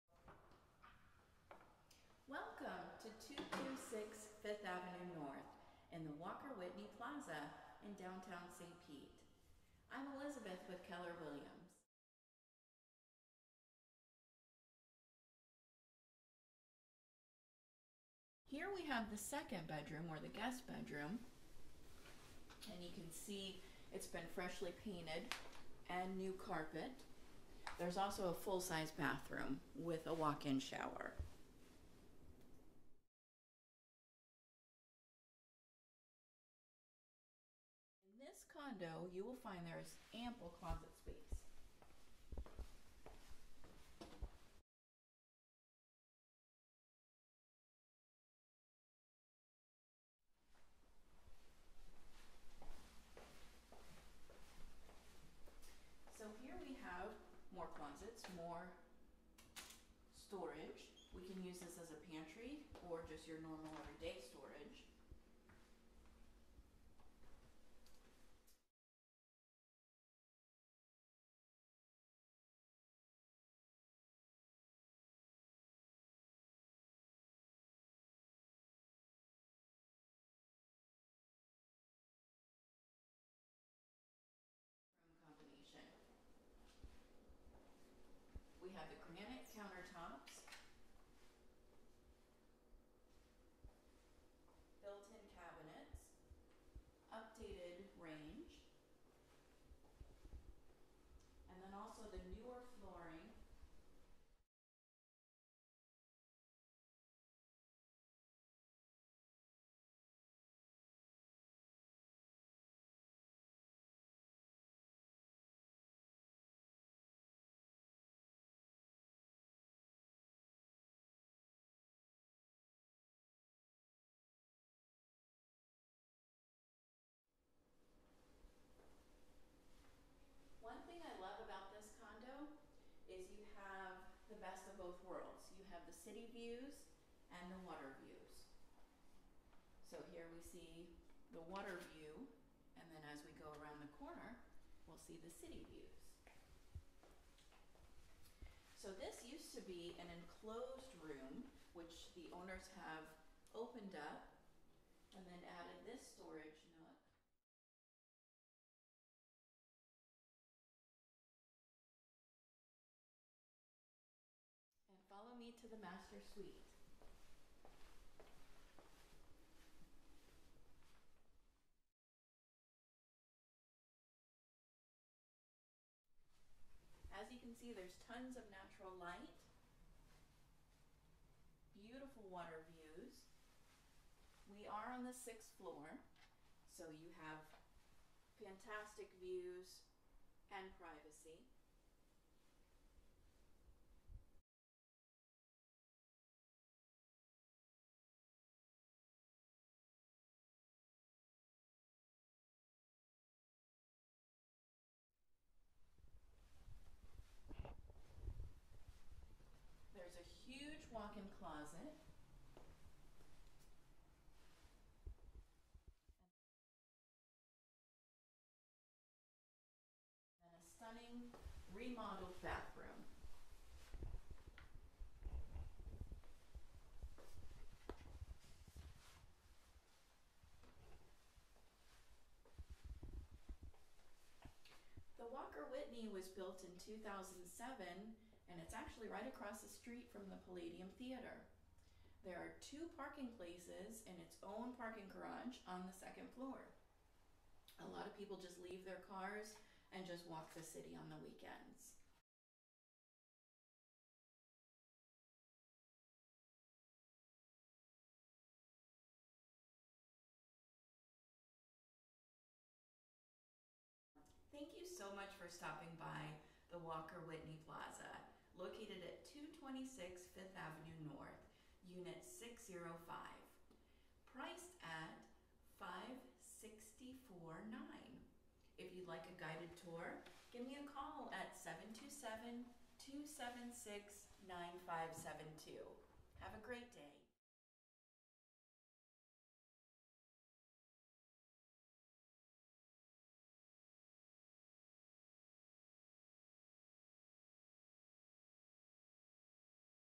226 5TH AVE N, #605, ST PETERSBURG, FL 33701
CONTEMPORARY, CHIC light and bright Downtown High-Rise Condo only blocks to Beach Drive,restaurants and shops, Sundial, AMC Theater as well as Tampa Bay waterfront parks. Dali Museum, Museum of Fine Arts, Mahaffey Theater and much more. This END unit features include LARGE 1684 sq ft OPEN CONCEPT home, spacious kitchen, granite countertops, long breakfast bar, stainless steel appliances that opens onto the great room and dining room. Dining room has an etched glass wine closet/storage closet. It has TWO BEDROOMS W/ ENSUITE BATHS, DEN or Study, HALF BATH, inside laundry, parking for 2 cars and tons of storage in the condo. Amenities include ROOFTOP DECK With CITY AND WATER Views, Outdoor grilling,and exercise equipment. WALK SCORE IS 93. Walk or Rent bicycles nearby at curbsides to tour the City or waterfront. The Palladium across the street offers cabaret and jazz music, opera, plays and other events. Drive west on 5th Ave to quick entry to Rt. I-275 and 15 minutes to Gulf Beaches. GO NE to Tampa, Disney, Orlando or go North to Tarpon Springs and the Nature Coast or South to Ellenton Mall and Sarasota . Great location to enjoy all that the West Coast of Florida offers.